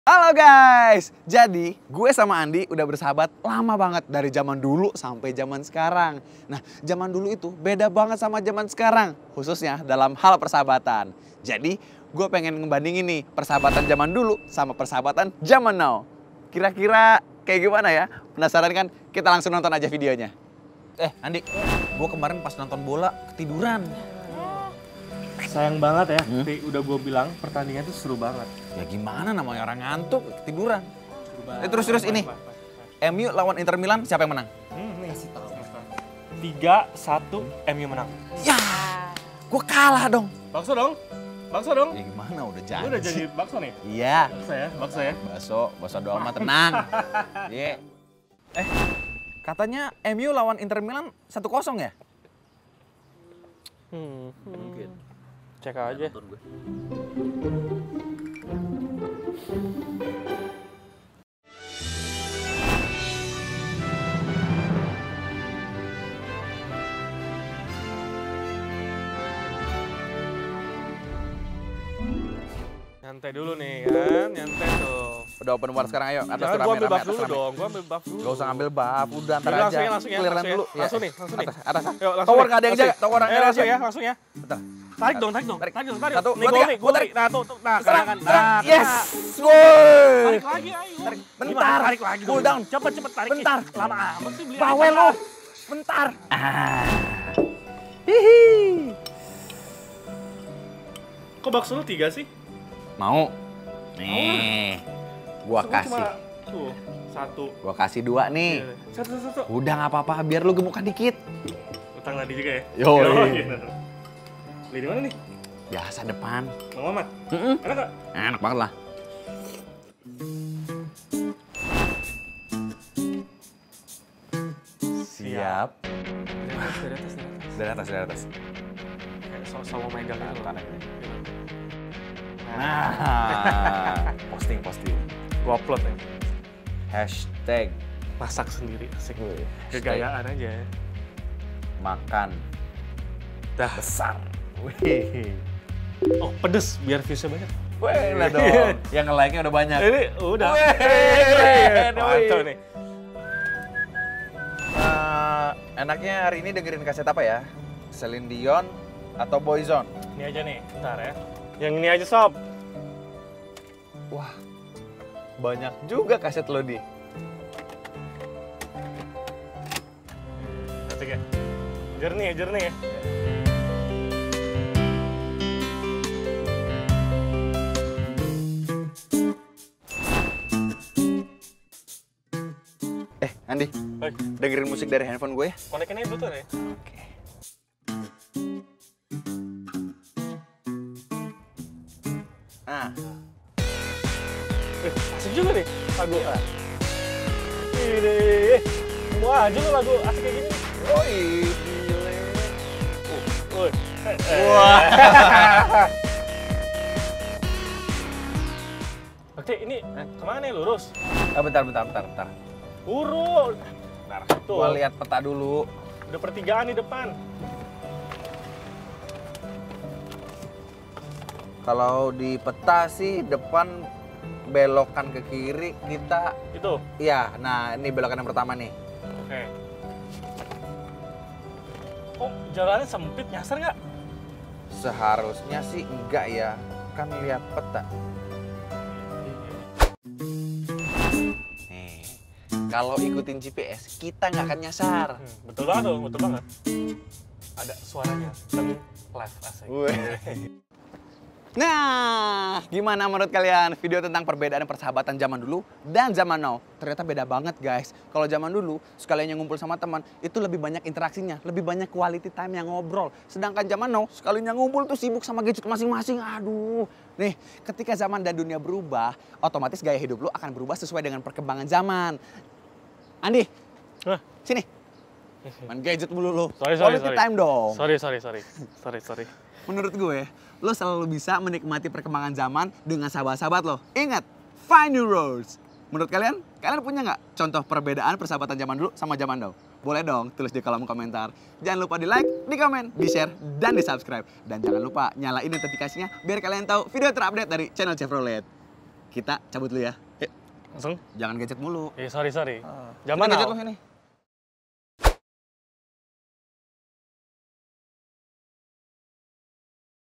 Beda Persahabatan Zaman Dulu VS Zaman Now, Kamu Pilih Mana?
Nggak percaya? Coba aja tonton video menarik bareng Bhakti ini dan share perbedaan persahabatan zaman dulu dan zaman now versi kamu!